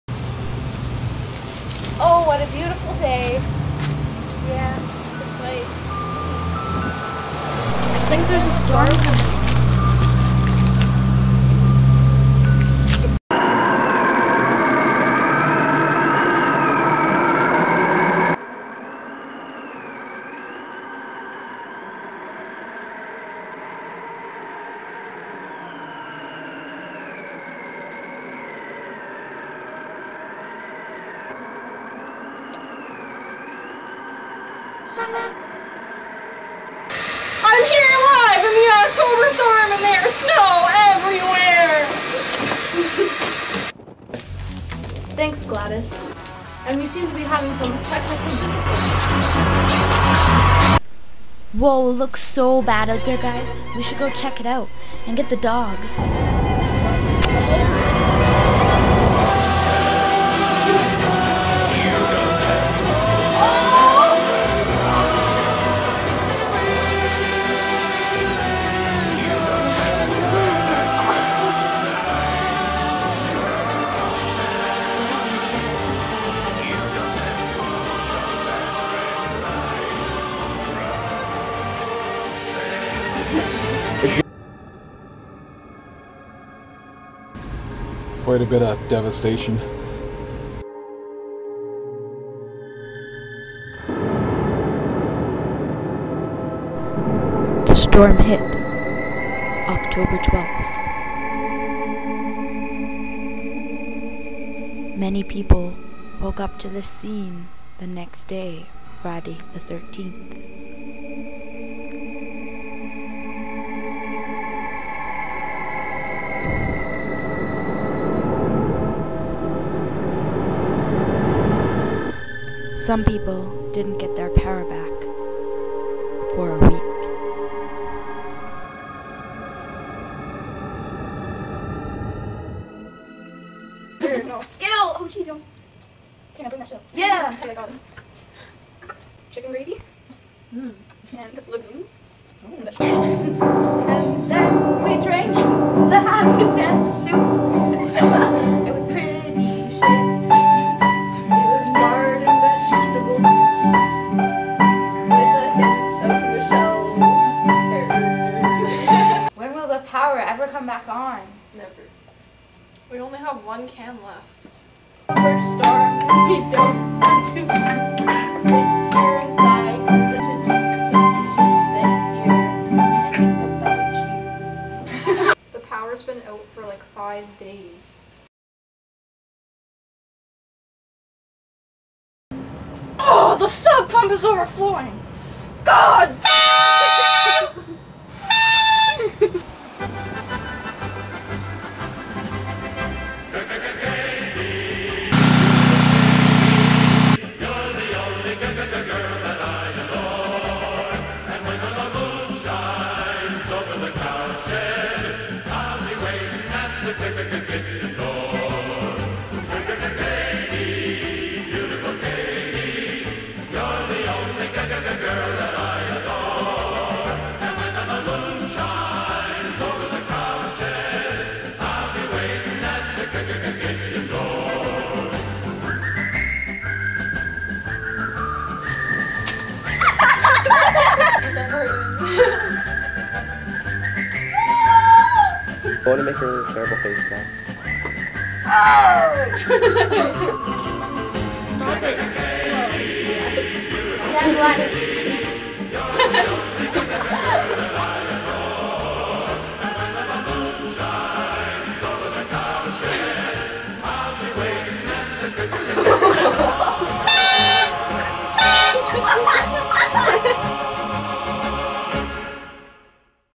Octoberstorm1youtube
Video we made for a class project on the OCtober storm in Fort Erie Canada/ Buffalo NY